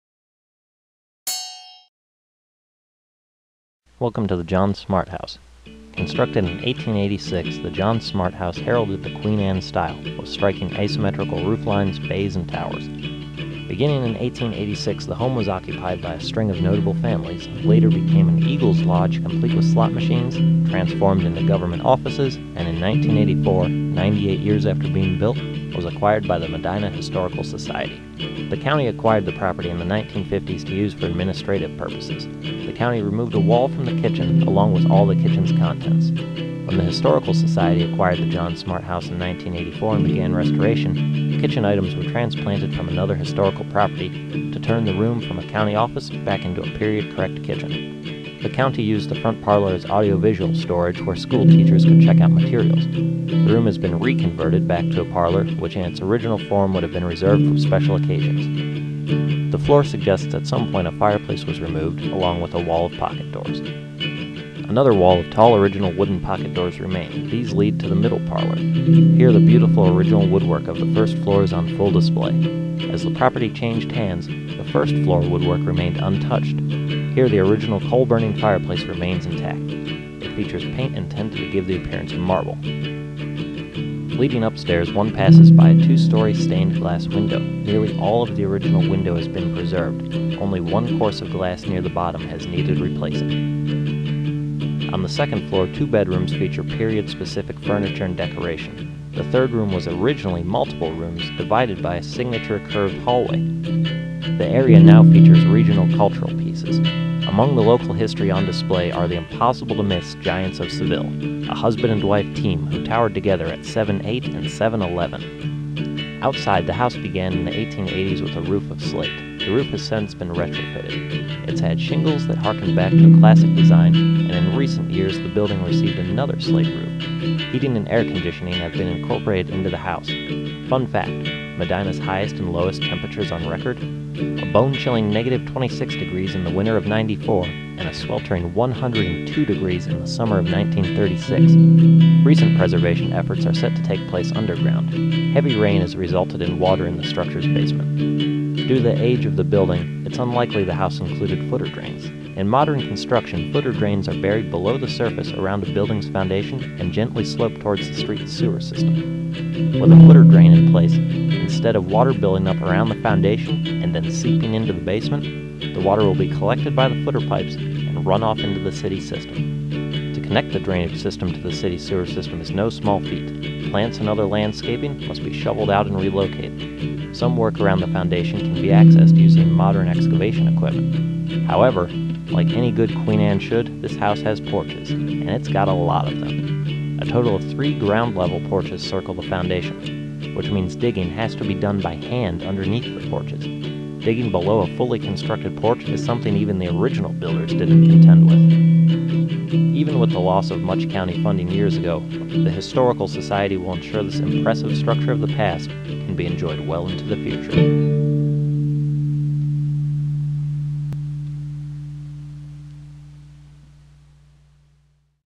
John Smart House
History reclaimed and restored at the John Smart House.
Submitted by: Chris Wetzel of Cleveland